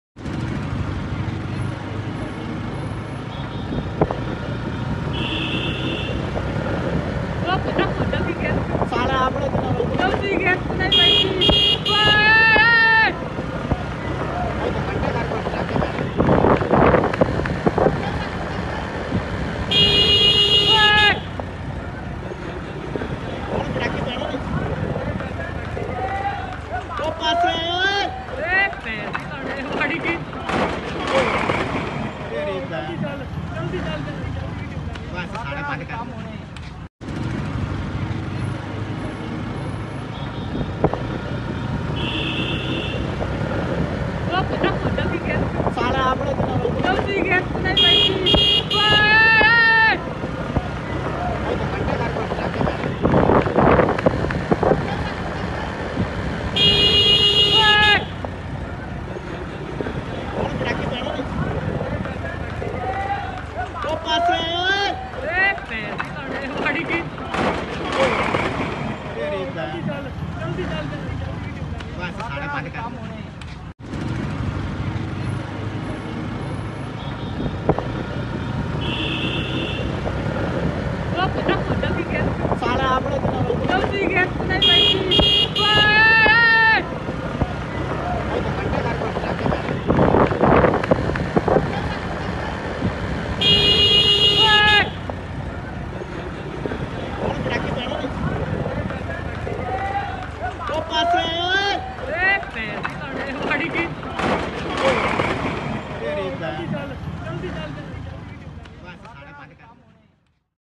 बिन ब्रेकां दी गड्डी ने मचाई तबाही
ईंटों से लदे रेहड़े का खच्चर बिदका
रेहड़े को बनाया जहाज, बड़ा हादसा टला